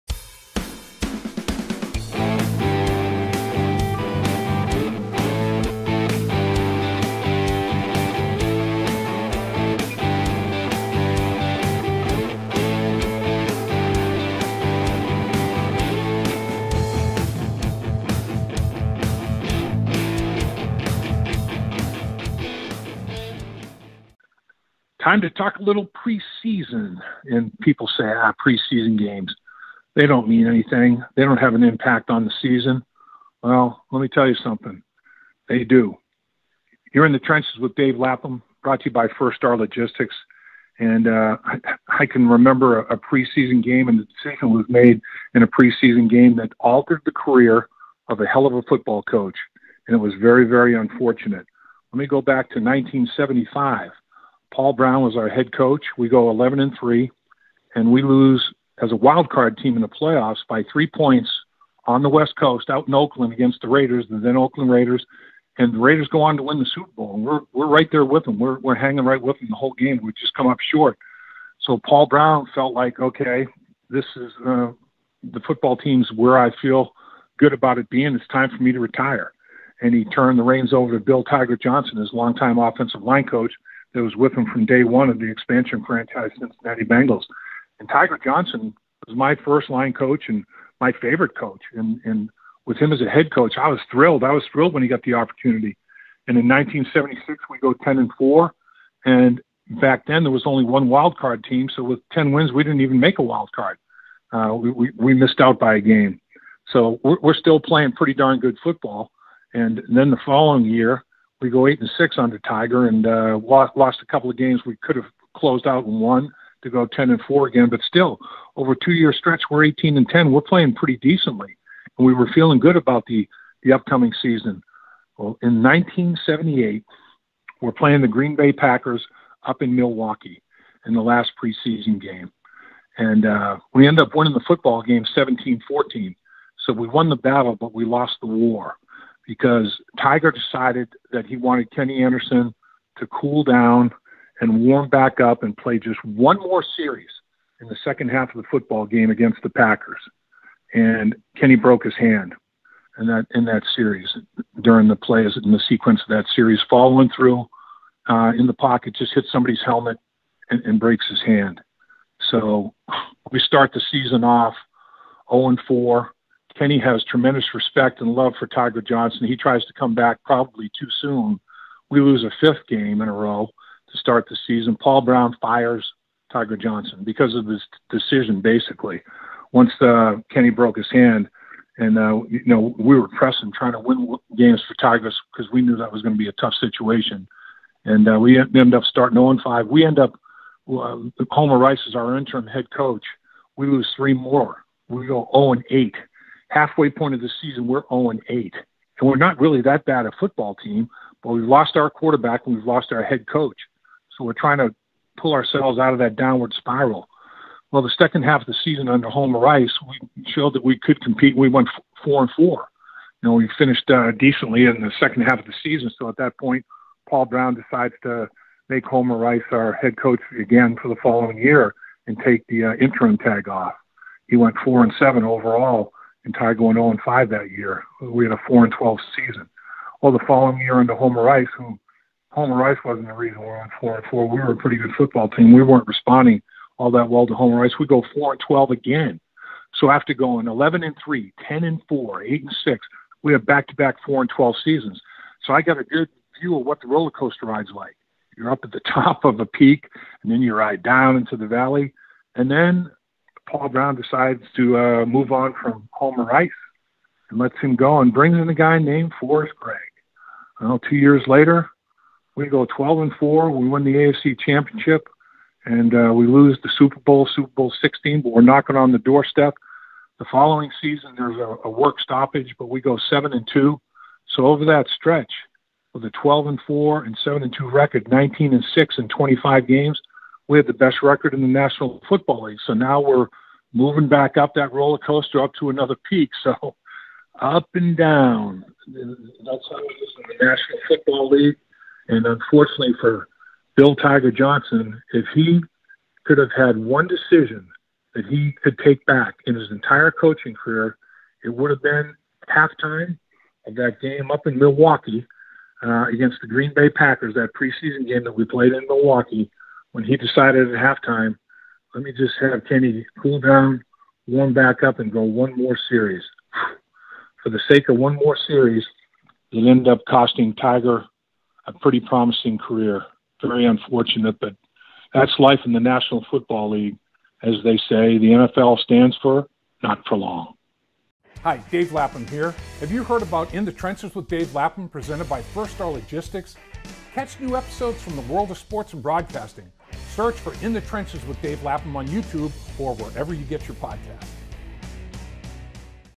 Dave Lapham Remembers How One Preseason Decision Changed Direction of the Bengals
Can a decision during the preseason have long-term effects on a franchise? In this episode of In The Trenches with Dave Lapham presented by First Star Logistics, Dave takes a look back at how one decision by then Bengals head coach Bill "Tiger" Johnson changed who was at the helm of the Cincinnati Bengals.